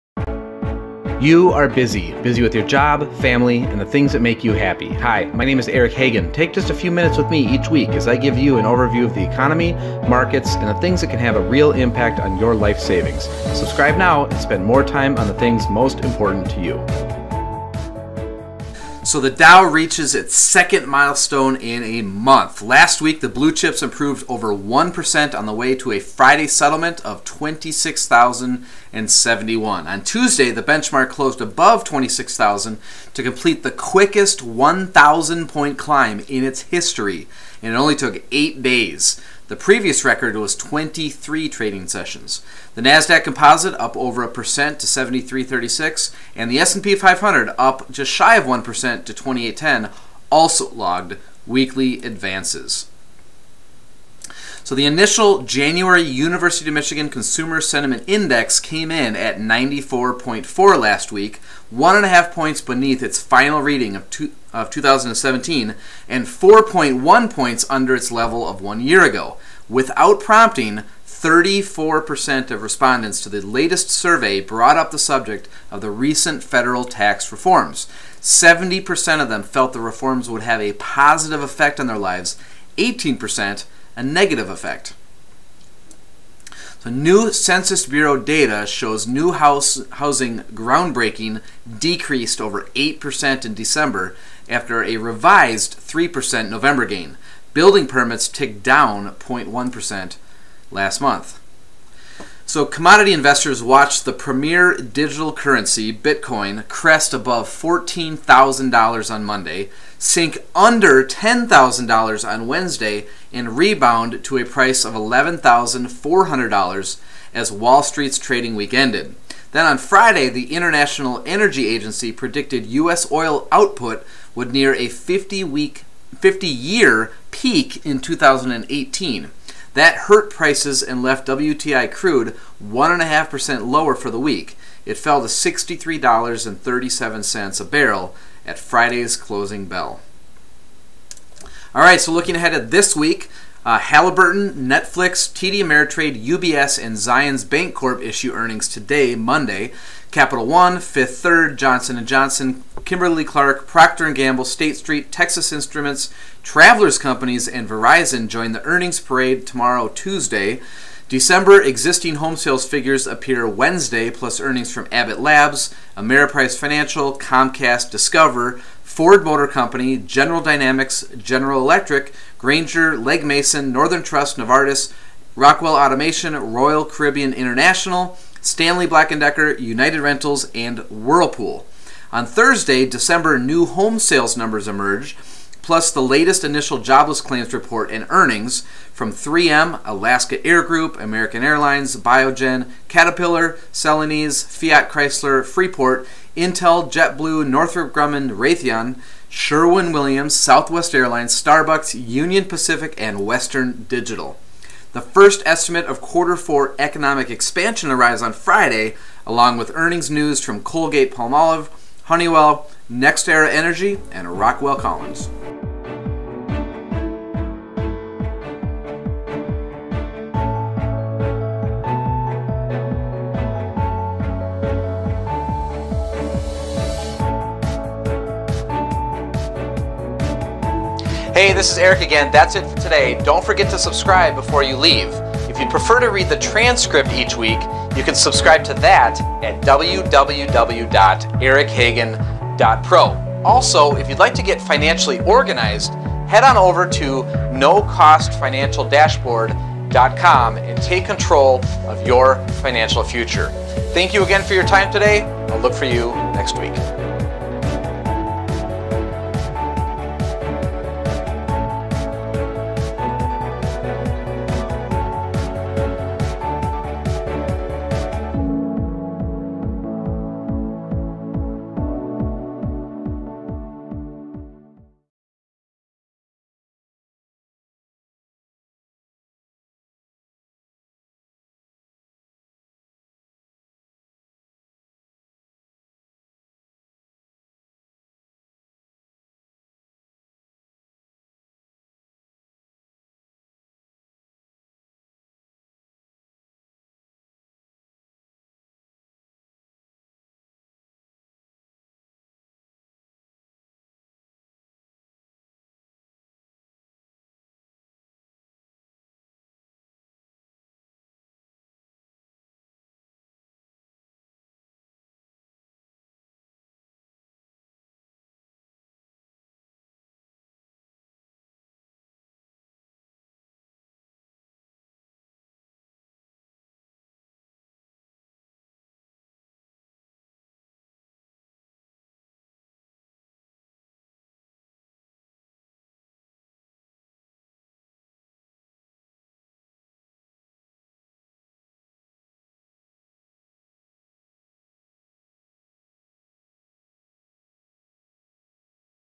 Weekly Economic Video Update for 1/22/2018
- DOW Reaches Its Second Milestone in a Month
- Consumer Sentiment Reading Cools
- Winter Weakens Housing Starts
- Bitcoin Plummets & Recovers; Oil Descends

► Did you know Eric is the father of 6?! Watch his story each day to get a behind-the-scenes glimpse of his crazy life